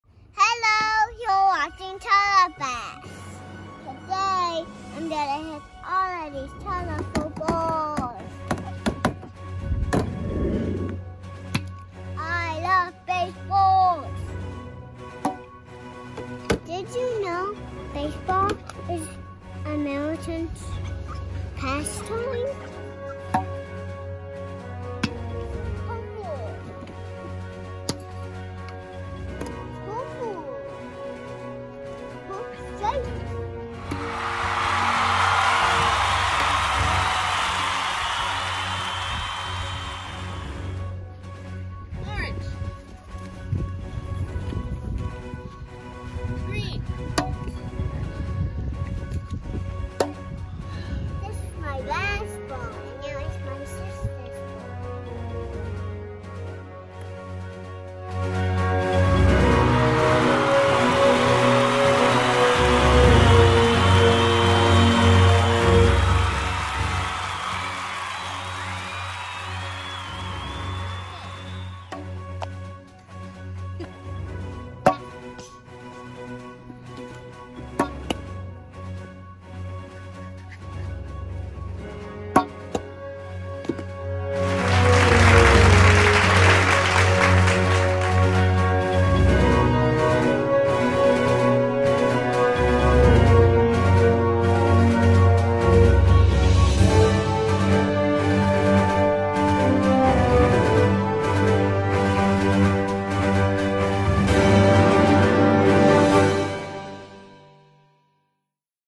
Color Ball Baseball
Bash and Gwen show you how to play color ball baseball. Enjoy home runs, strikeouts and Dandy in the infield!